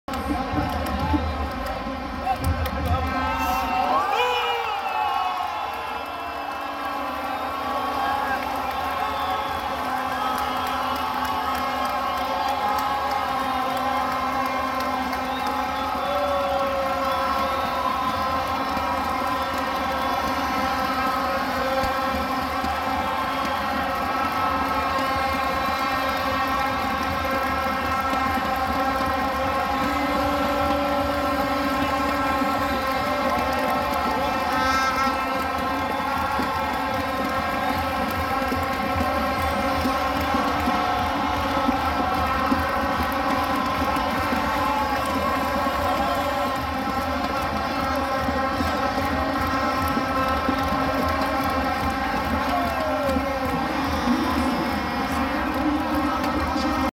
Mohamed Salah Goal vs Uganda AFCON 2019 هدف محمد صلاح في مرمي أوغندا
Mohamed Salah goal against Uganda in the African Cup of Nations 2019 hosted by Egypt